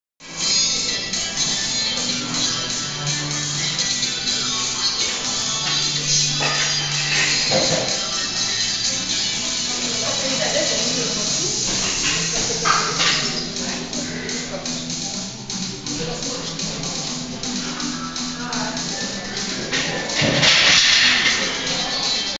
Тренировка прыжка.Присед с колен 220 кг
3 цикл тренировок прыжка подошли в плотную к конечной фазе!